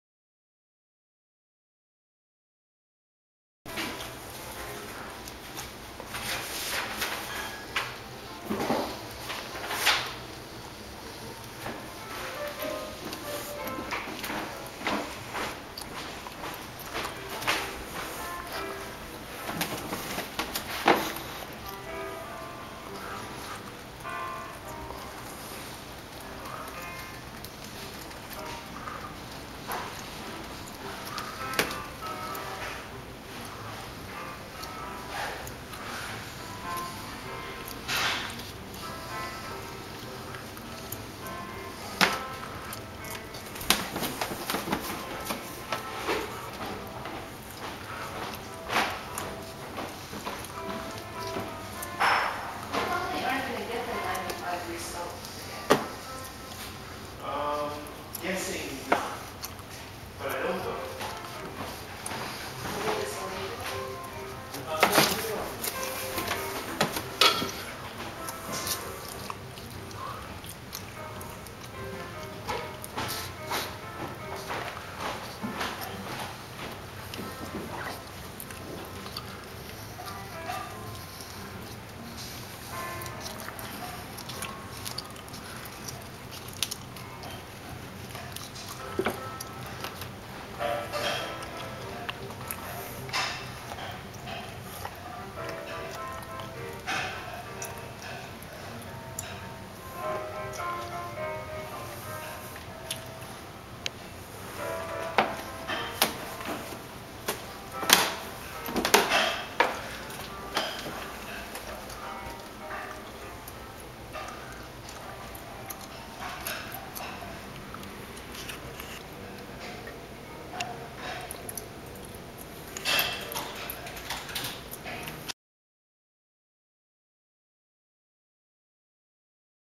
]twenty-six paces[
Drawing made on the floor of Artisan Wine Shop in Beacon, NY while installing Telephone for the WIndows on Main St. exhibit.  August 13, 2011.

This video also demonstrates that I can do at least three things at the same time: make a video (shakily), draw a line (not straight), and chew gum (but not silently) - as you can hear.